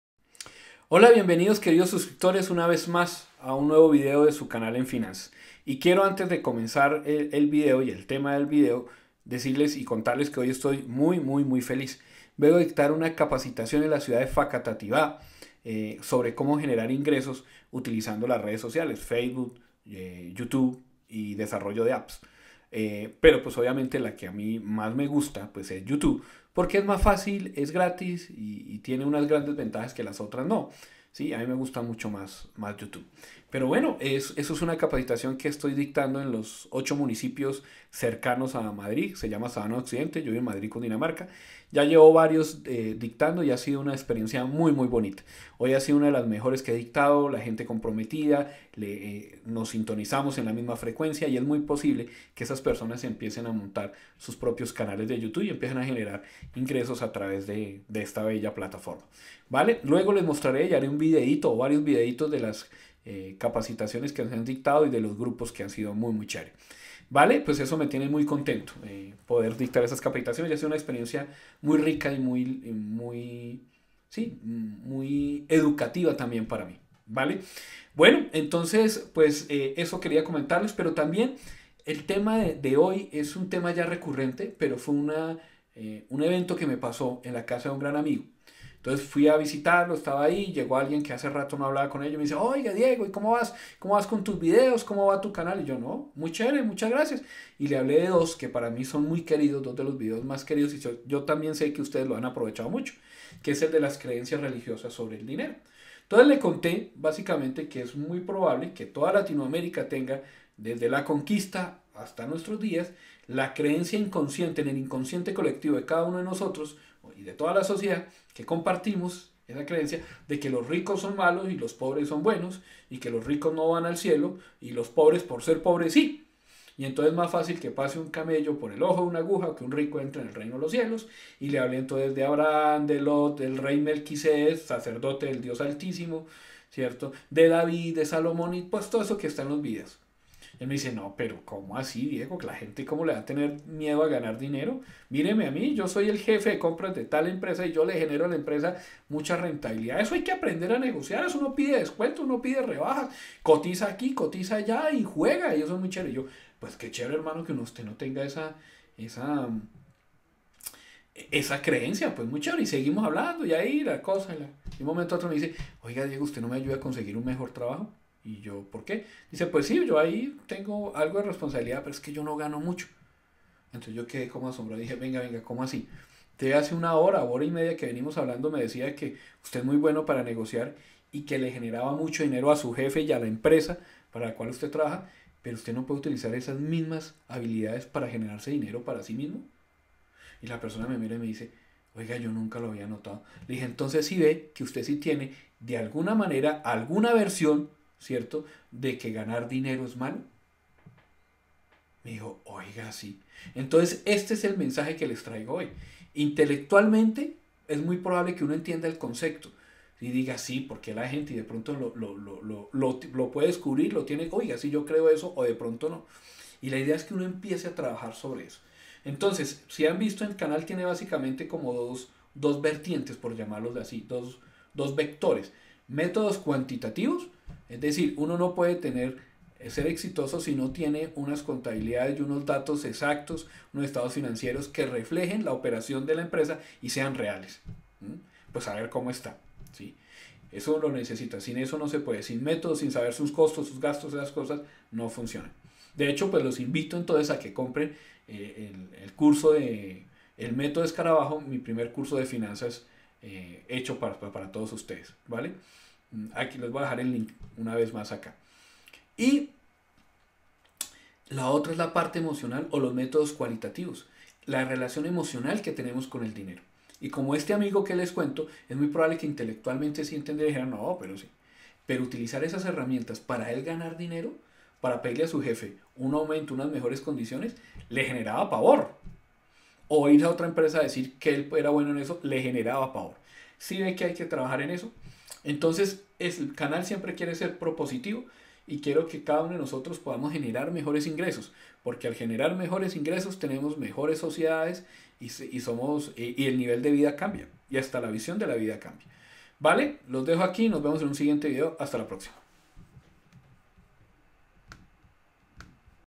PEQUEÑA REFLEXION, ¿COBRAS LO QUE VALES? NO SABES COBRAR.
A veces nos es fácil generar ingresos económicos para otras personas pero para nosotros mismos NO ES IMPOSIBLE!!!
Debemos desarrollar las habilidades necesarias para generar ingresos para nosotros. No es egoísmo, es amor propio.
¿Generas grandes cantidades de dinero para tu empresa, pero te aterra negociar un aumento con tu jefe?
 NO SABES COBRAR LO QUE VALES!! Tienes talento, eres cumplido pero no ganas porque no cobras bien!!